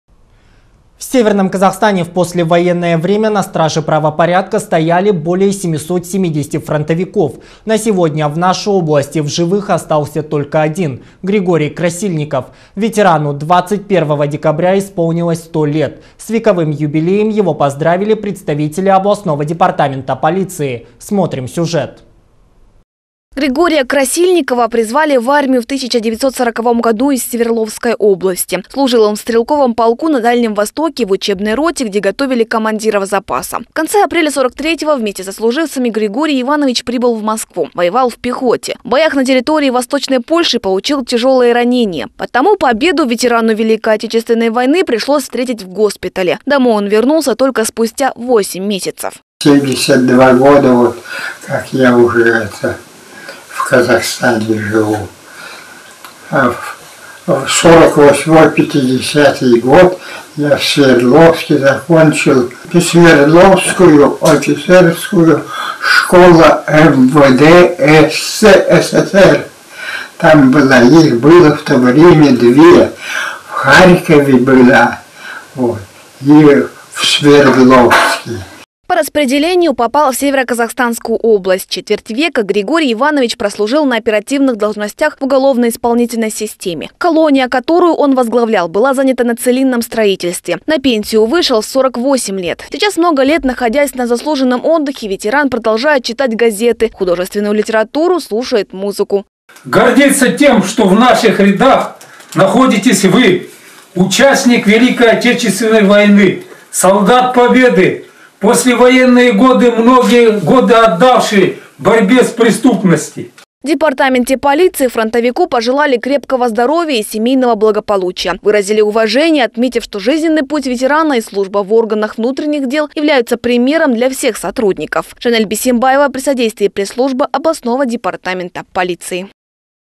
100-летний юбилей отмечает ветеран ВОВ Григорий Красильников
В СК в послевоенное время на страже правопорядка стояли более 770 фронтовиков. На сегодня в нашей области в живых остался только один - Григорий Красильников. Ветерану 21 декабря исполнилось 100 лет. С вековым юбилеем его поздравили представители областного департамента полиции. Смотрим сюжет.